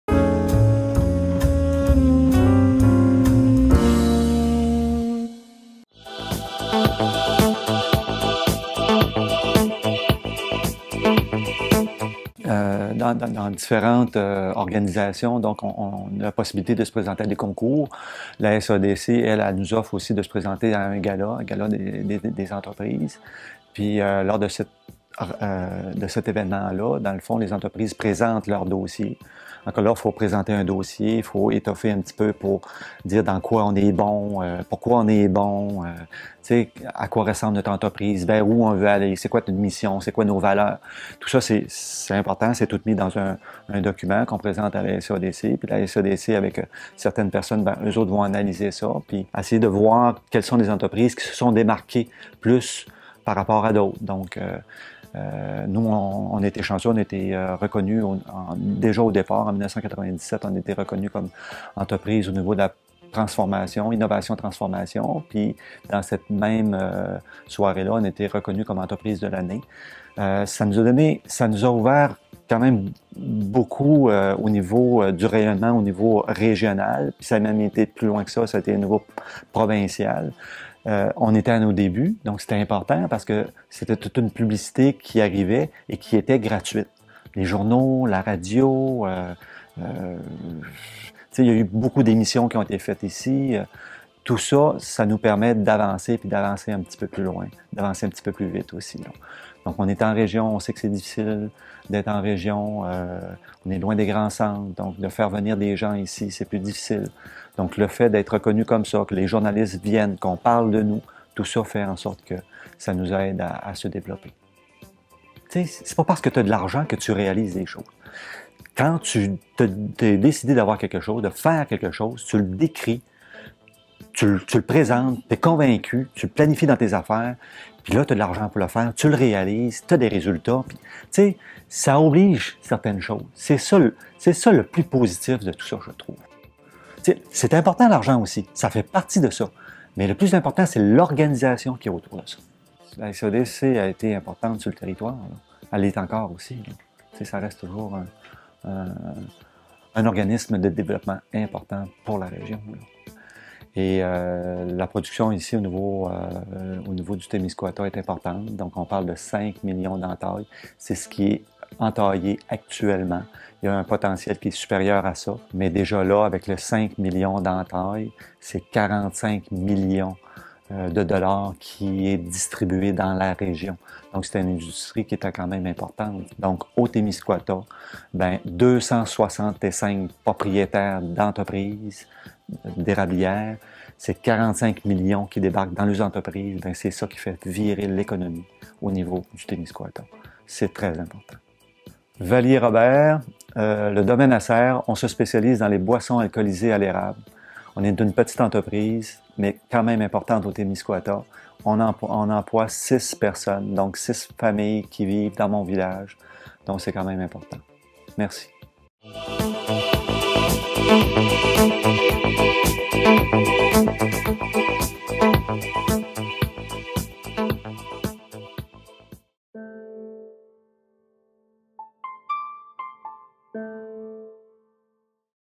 Témoignage : Vallier Robert
Une vidéo réalisé par WebTémis et produite par la SADC de Témiscouata dans le cadre de son trentième anniversaire, témoignage d'entrepreneur avec Vallier Robert du Domaine Acer. (vidéo 4)